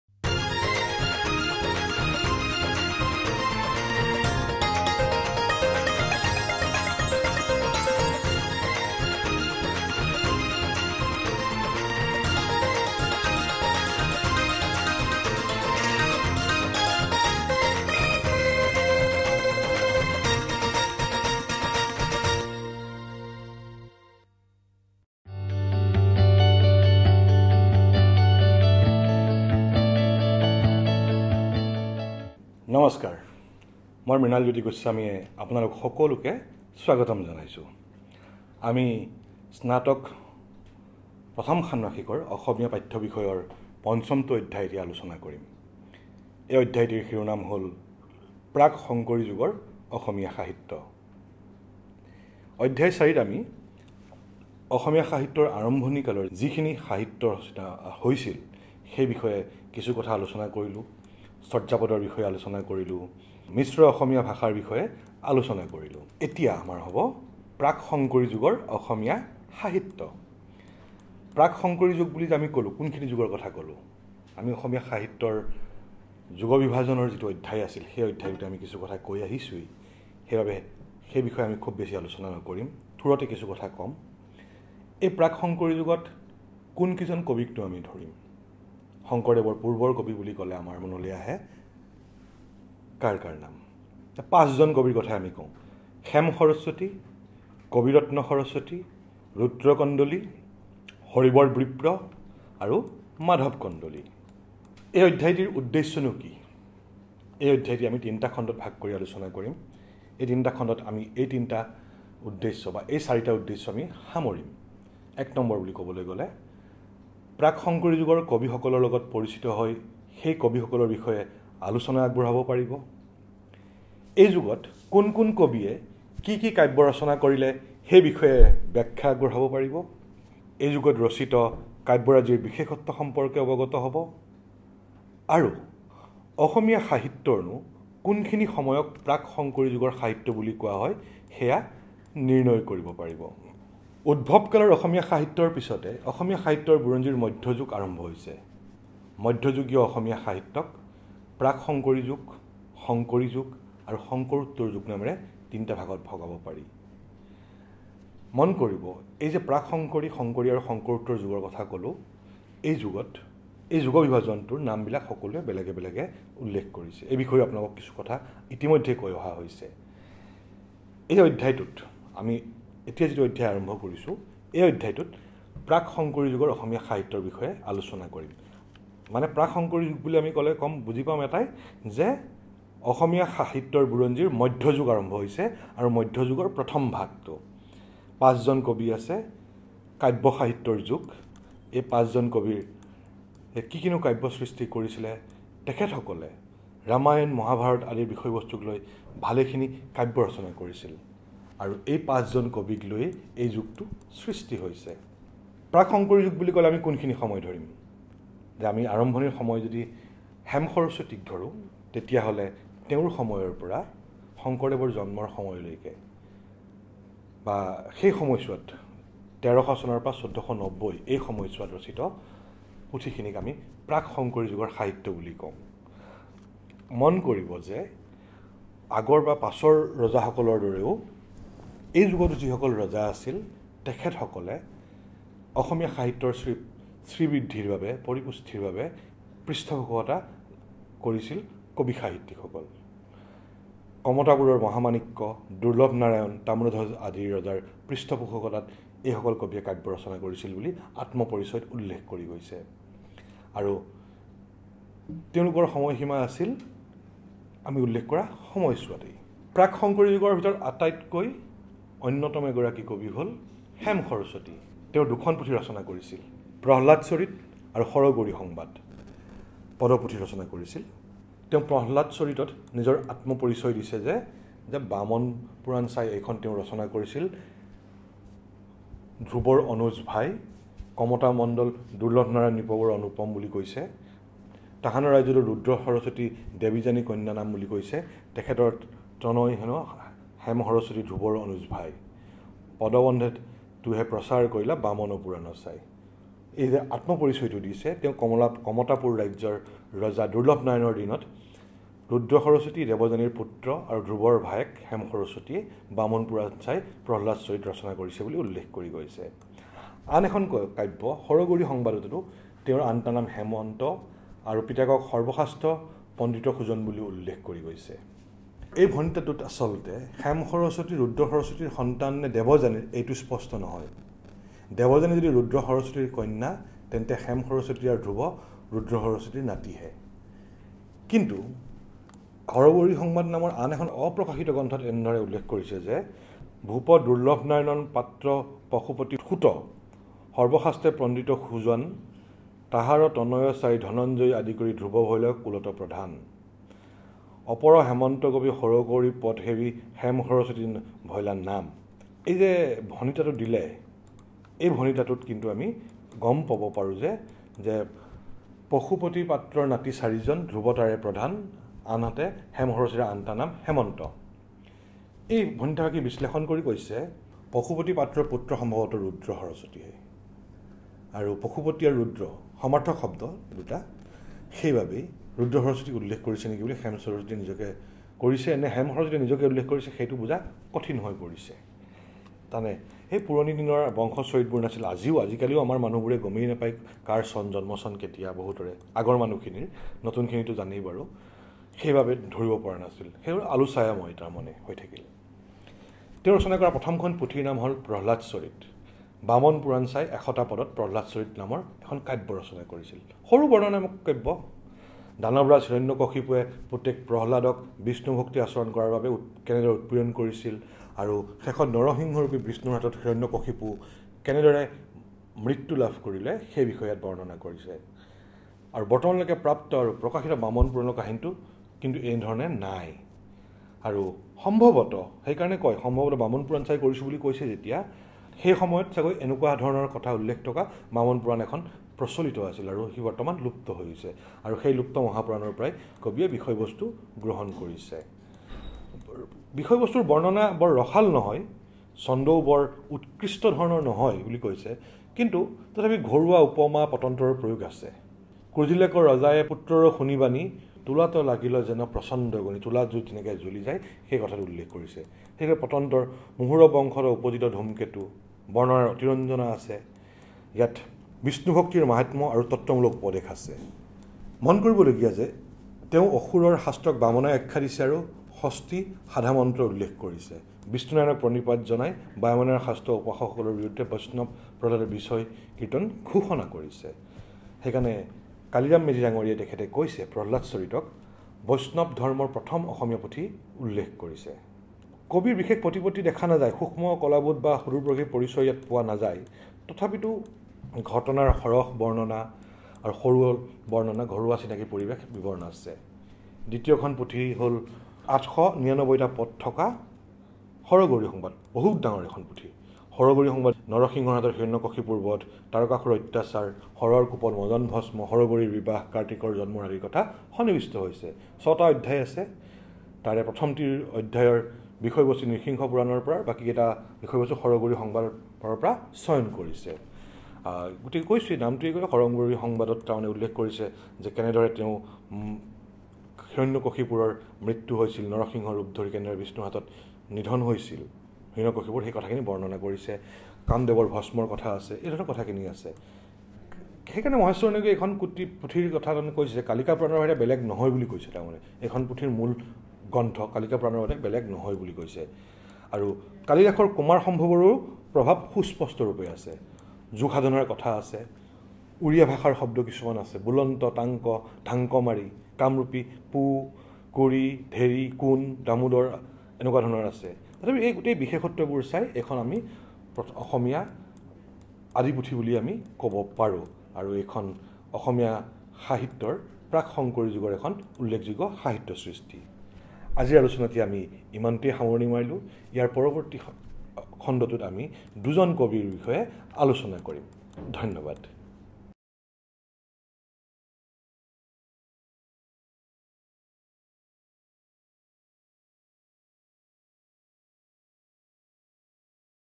Course:1_Unit-5: Prak Shankari Jugar Axomiya Sahitya (Part-1)
Presented By: Dr. Mrinal Jyoti Goswami, Assistant Professor (Assamese), Padmanath Gohain Baruah School of Humanities, KKHSOU